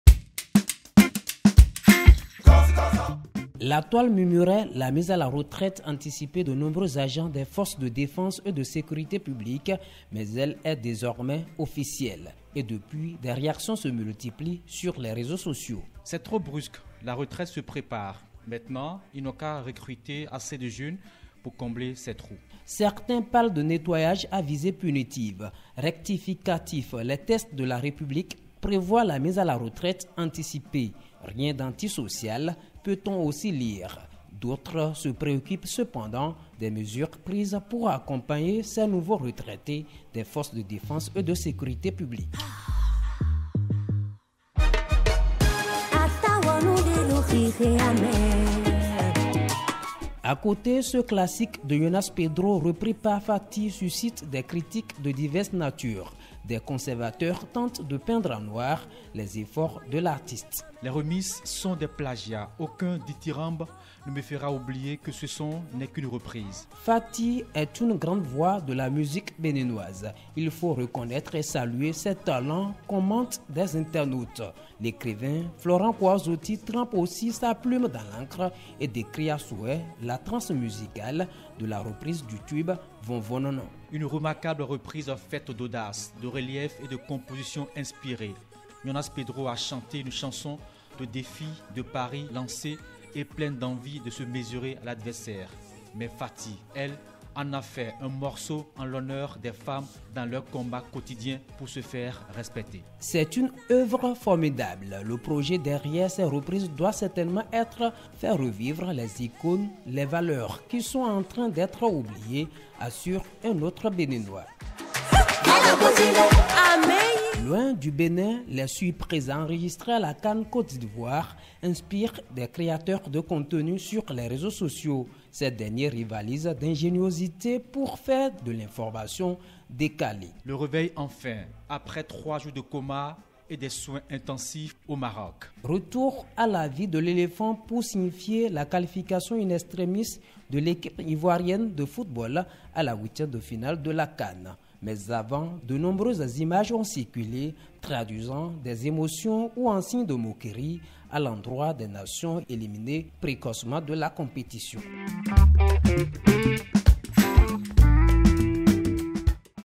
Echos du web : les buzz de la semaine du 22 janvier 2024
Retrouvez ici les sujets qui ont fait réagir sur la toile du 22 au 28 janvier 2024.

Au menu des échos du web, la mise à retraite anticipée de nombreux agents des Forces de défense et de sécurité publique, le regard critique sur le nouveau morceau de l’artiste Faty et les surprises de la CAN Côte d’Ivoire en images.